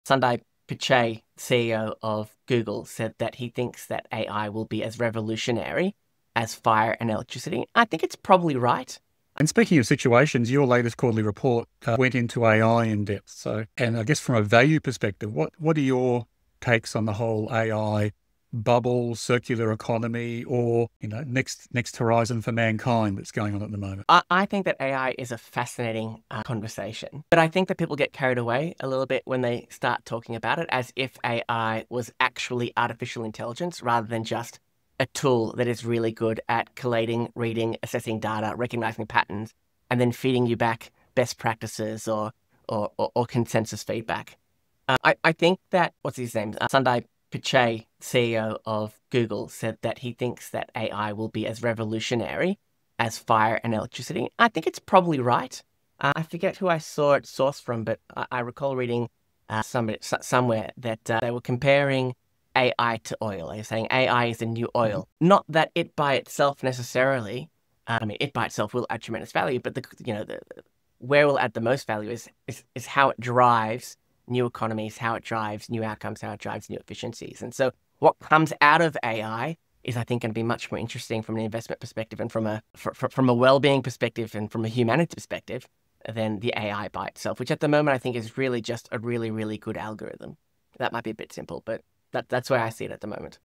AI's Impact: Google's CEO on the Future of Innovation
Google's CEO sees AI transforming the world like fire or electricity. But is it true intelligence? Or just a powerful tool for insights and efficiency? The real value lies in the economies and outcomes AI creates.